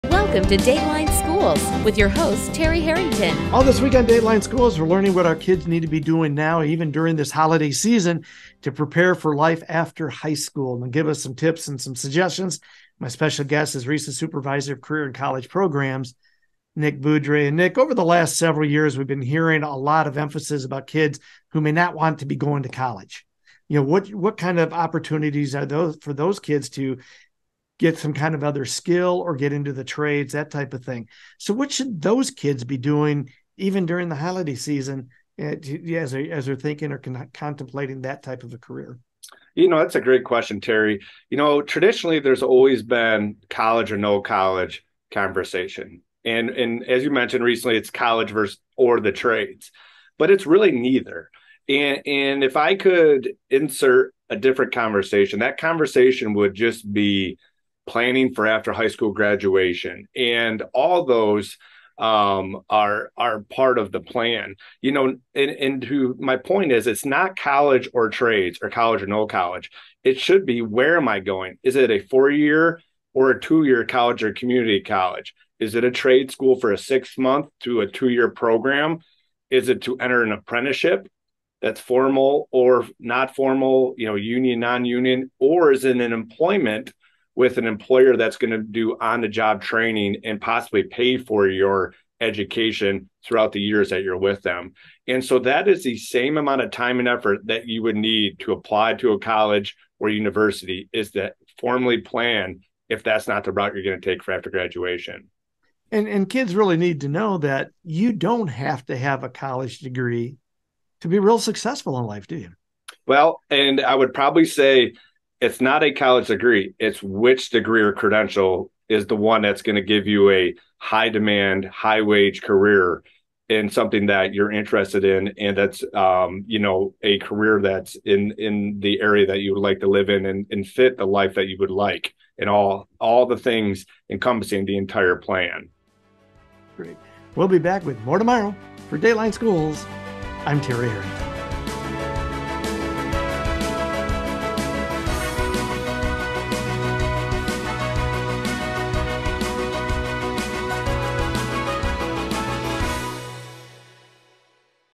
Dateline: Schools - FASFA - Thursday
RESA's Supervisor of Career and College Programs, Nick Beaudry, talks with Terry about the importance of completing FAFSA applications.